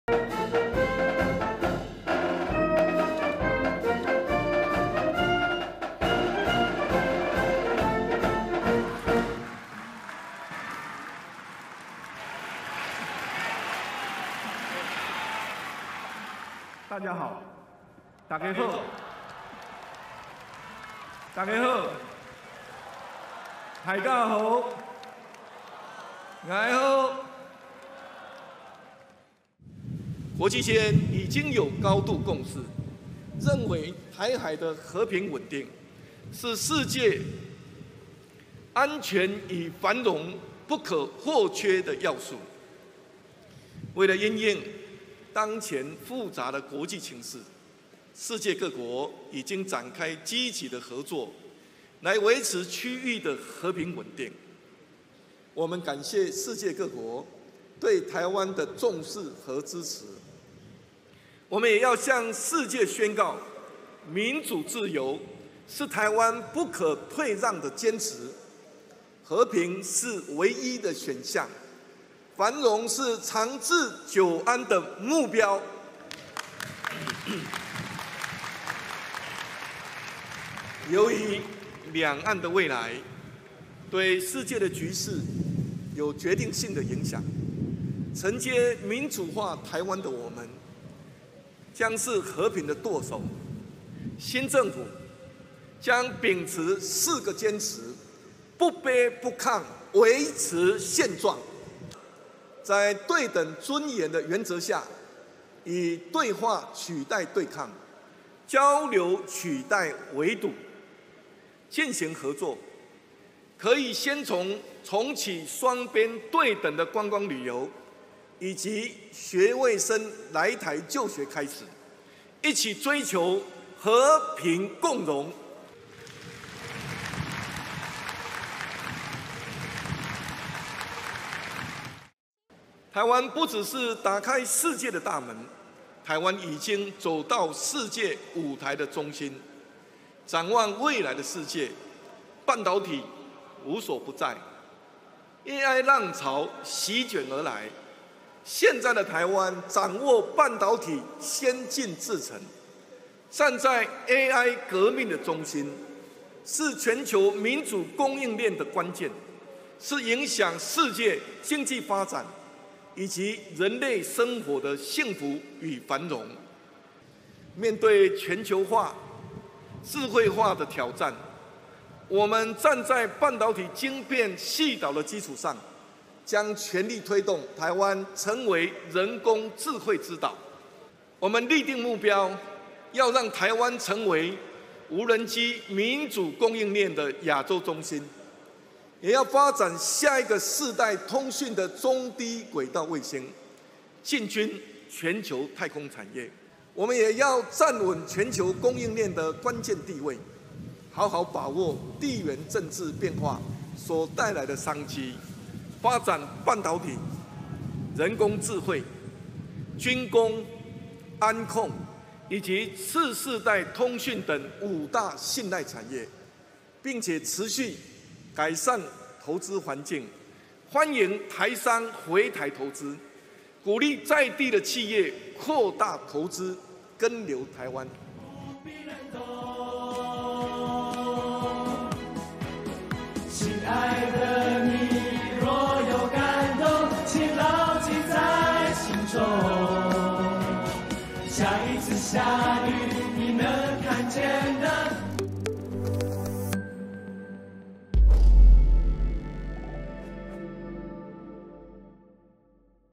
赖清德就职演说：不卑不亢维持现状
台湾新任总统赖清德强调，新政府将秉持“四个坚持”，不卑不亢，维持现状，并呼吁中国大陆停止对台湾文攻武吓，正视“中华民国”存在的事实，拿出诚意，和台湾民选合法的政府在对等、尊严的原则下进行合作。
立即加入《联合早报》Telegram和WhatsApp频道，获取即时新闻推送，方便掌握最新消息。